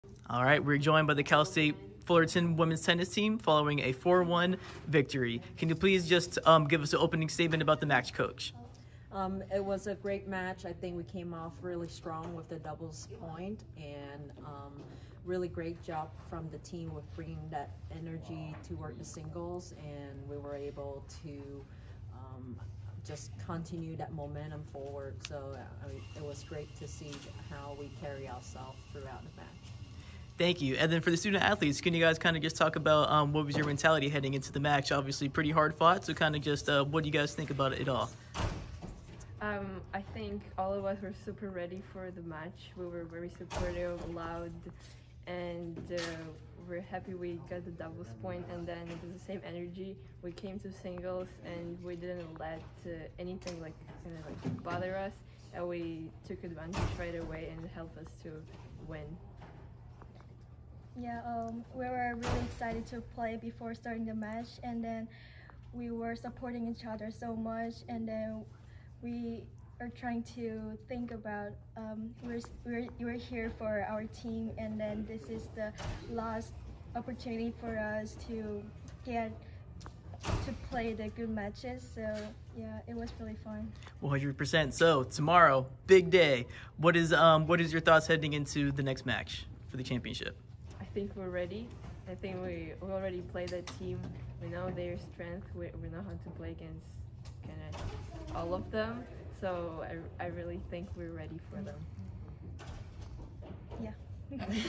2024 Big West Women's Tennis Championship: No. 6 Cal State Fullerton
Cal State Fullerton head coach Trang Huynh-McClain and student-athletes Seha Yu and Mariia Nikitash speak after No. 6 seed Cal State Fullerton defeated No. 7 UC Davis, 4-2 in the semifinal round of The 2024 Women's Tennis Championship at Barnes Tennis Center in San Diego, Calif.